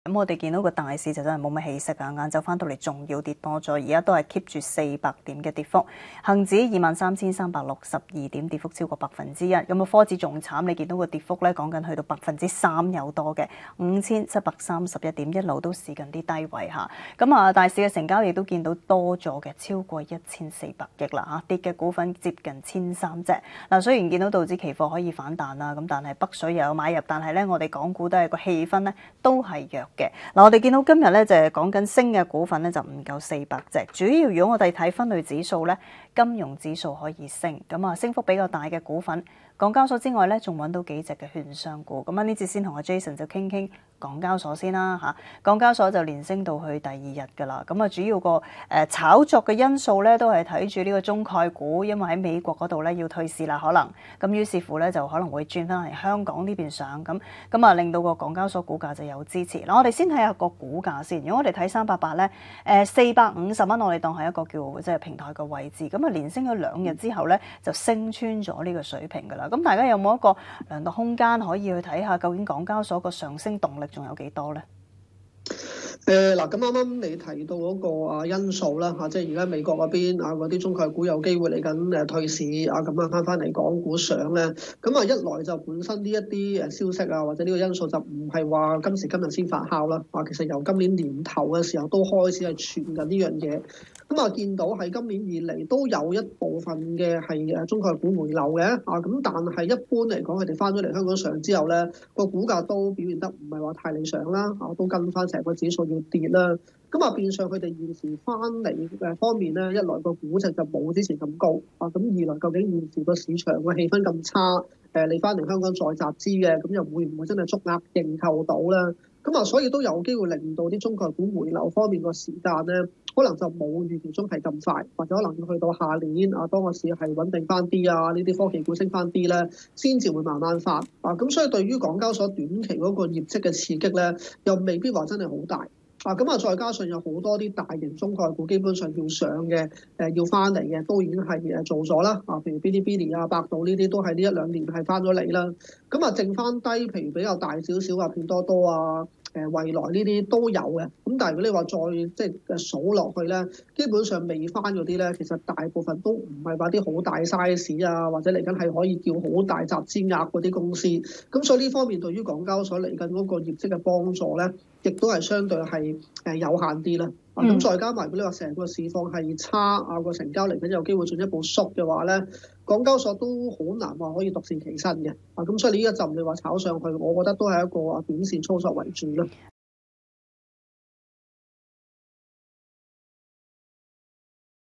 【專家分析】港交所呢浸升浪短癮？
【Now財經台】港交所(0388)連升第2個交易日，市場憧憬陸續有喺美國掛牌中概股「棄美來港」，帶挈港交所股價升穿450元水平，上返平台區，股價仲可以走得幾遠？
一齊聽吓 uSmart盈立證券研究部總監 陳偉聰 點講！